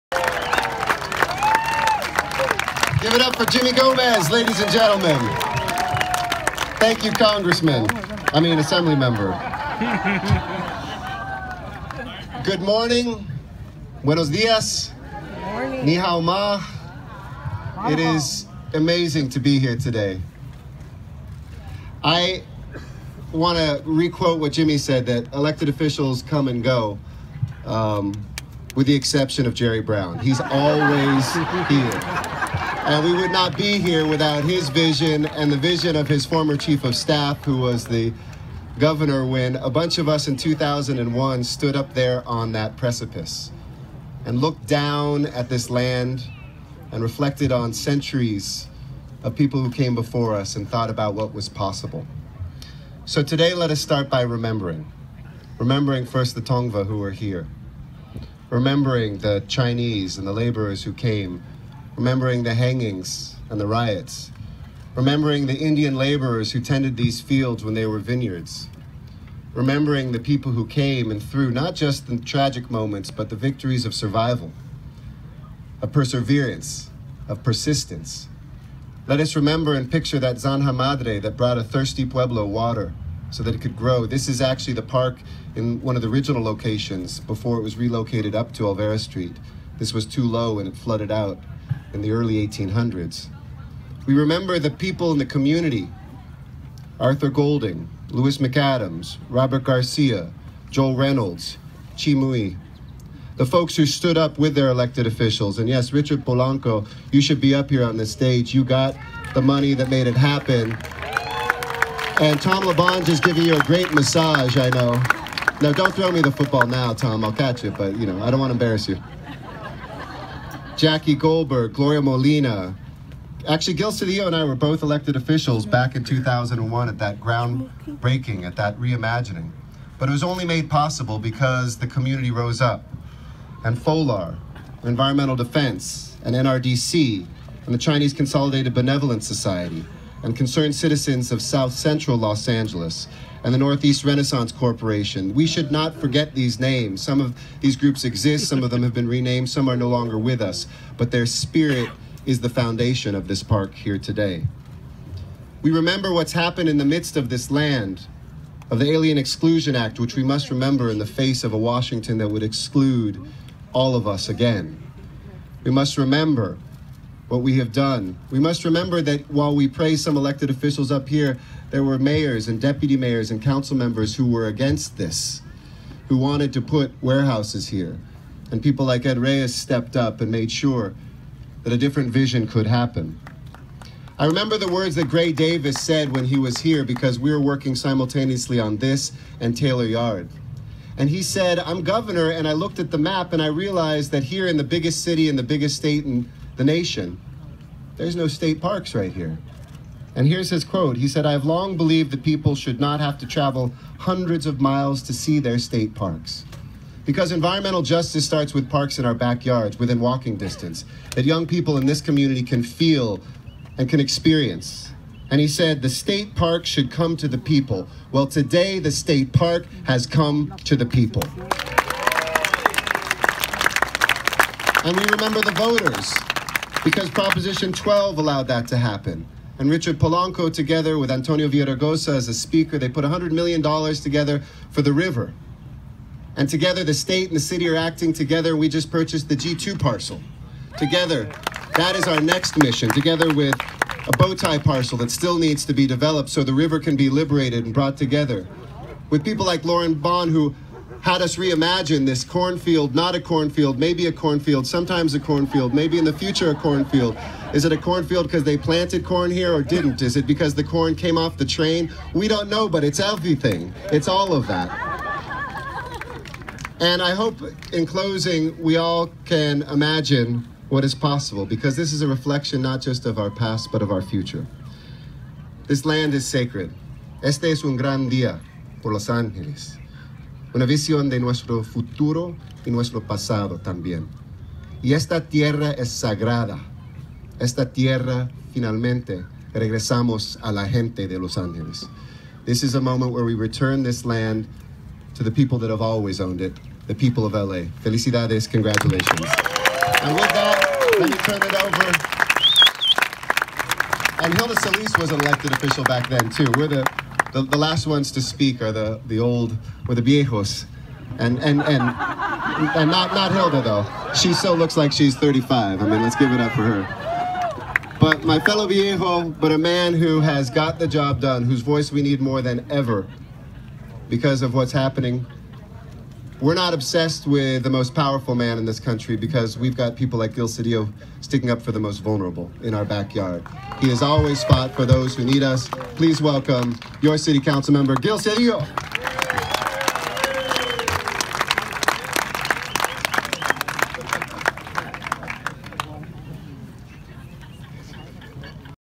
Mayor Eric Garcetti's Speech at the Inauguration of the Los Angeles State Historic Park
Mayor Eric Garcetti's Speech at the Inauguration of the Los Angeles State Historic Park (Saturday, April 22, 2017)